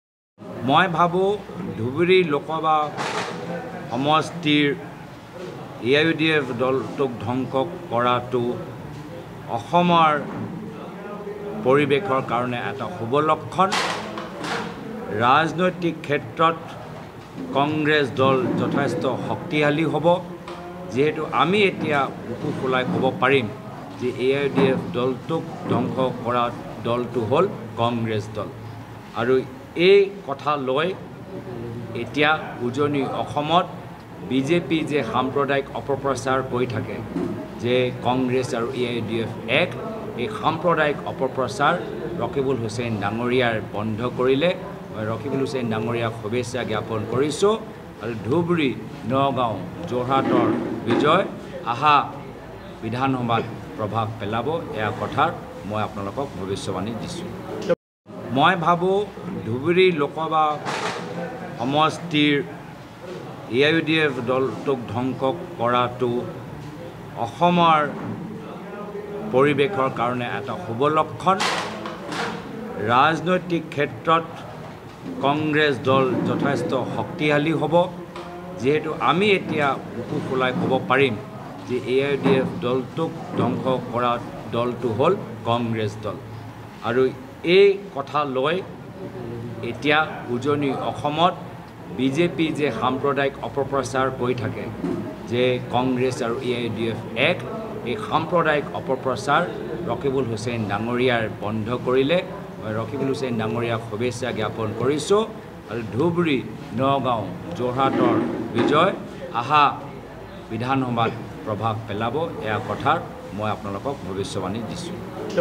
Gaurav Gogoi : ৰাজনৈতিক ক্ষেত্ৰত কংগ্ৰেছ দল যথেষ্ট শক্তিশালী হ'বঃ গৌৰৱ গগৈ | N18V
Gaurav Gogoi | ধুবুৰী সমষ্টিত AIUDF দলক ভংগ কৰাটো অসমৰ পৰিৱেশৰ বাবে শুভ লক্ষণ । ৰাজনৈতিক ক্ষেত্ৰত কংগ্ৰেছ দল যথেষ্ট শক্তিশালী হ'বঃ গৌৰৱ গগৈ